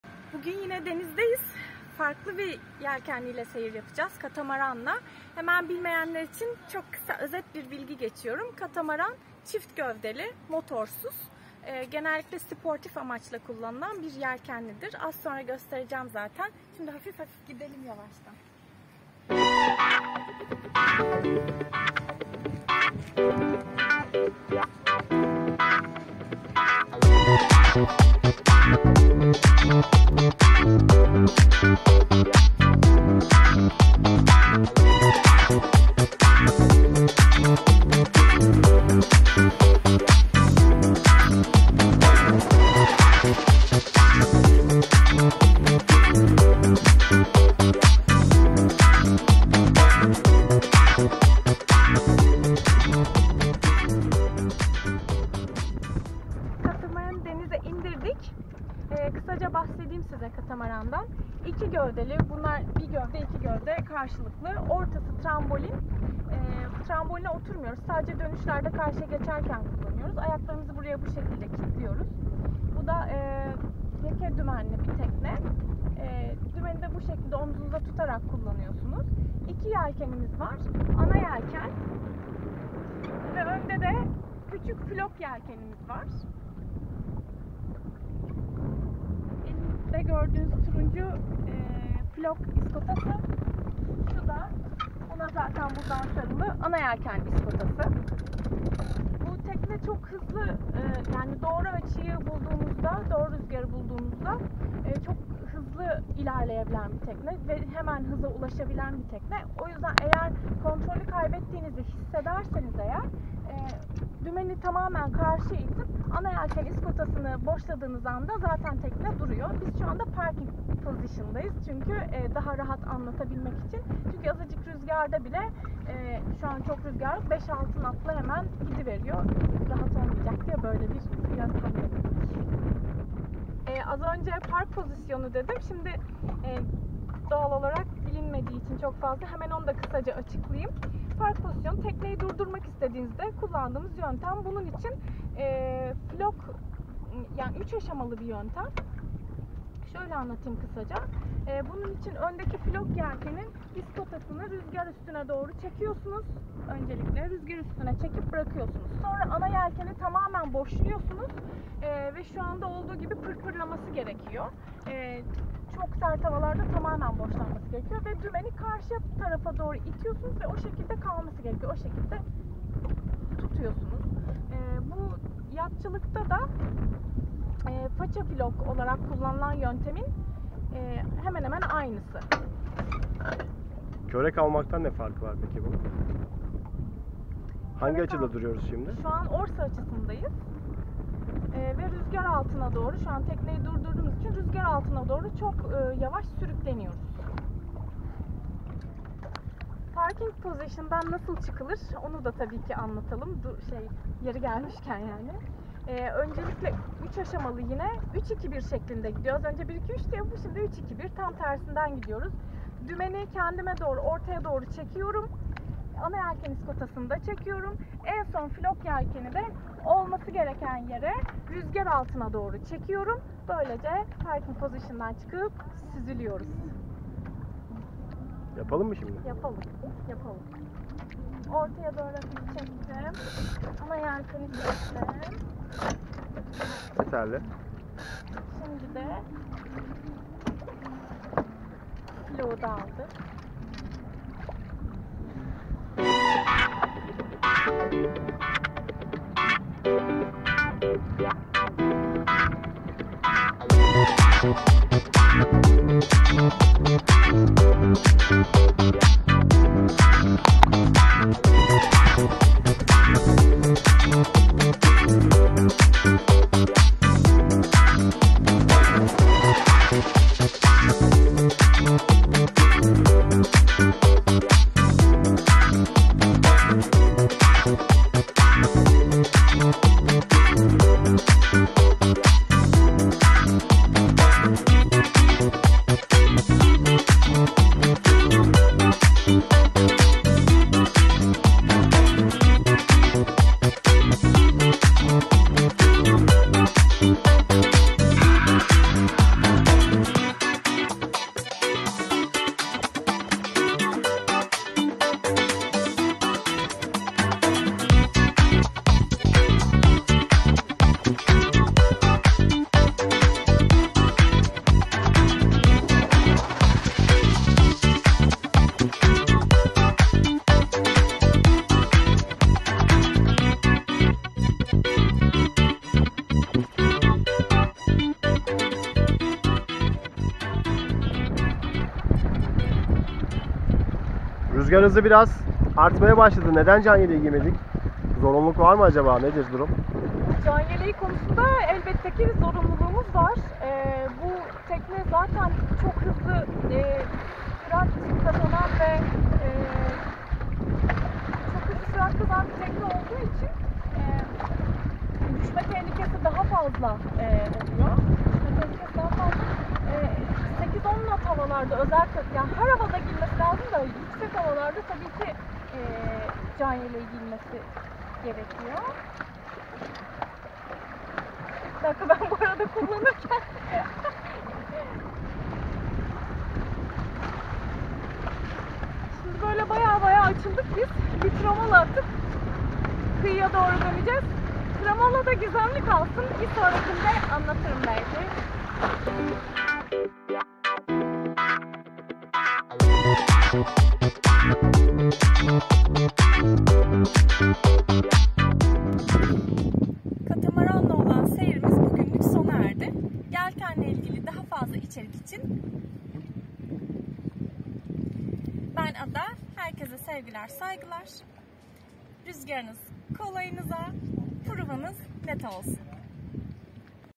Katamaran seyri/Katamaran nasıl kullanılır
Merhabalar...
Bu videoda Topcat K3 katamaran ile denize açılıyoruz.
Katamaran Eğitimi içeriği ile ilgili biraz bilgi verdiğimiz bu video hem eğlenceli hem de heyecanlı bir katamaran deneyimi oldu.
Katamaran eğitimiyle ilgili yeni videoları yakında kanalımızda paylaşmaya devam edeceğiz.
İyi seyirler